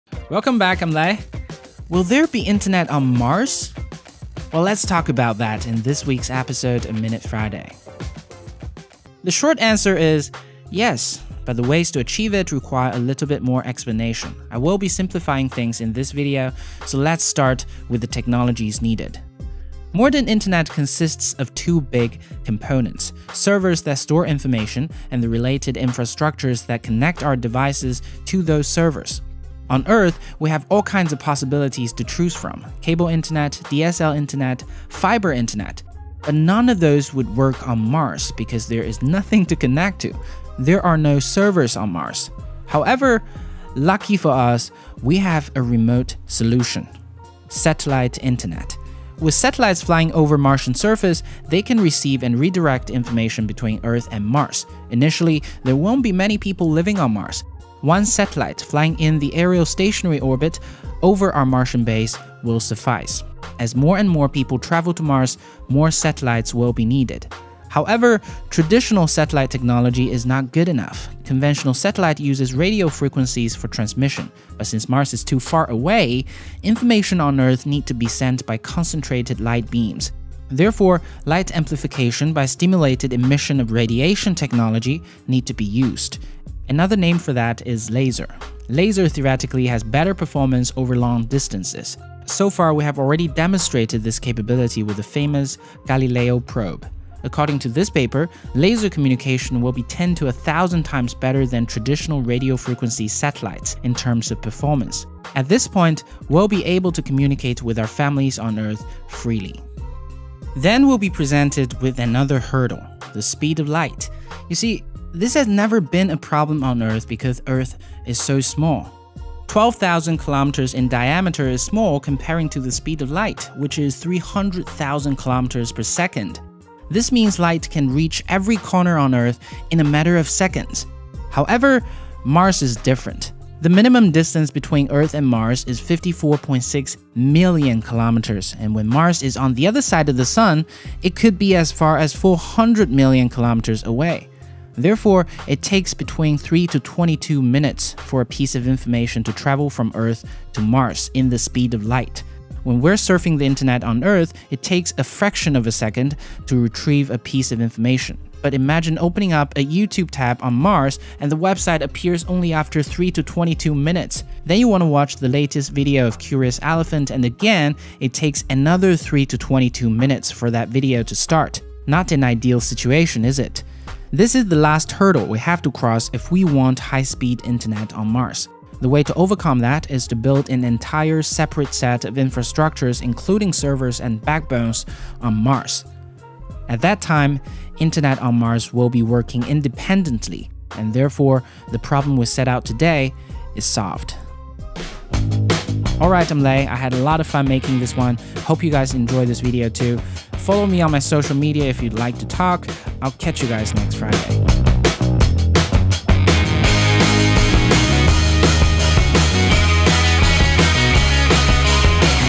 Will there be Internet on Mars?
Yes, we will have internet on Mars, but it's gonna be slow at first.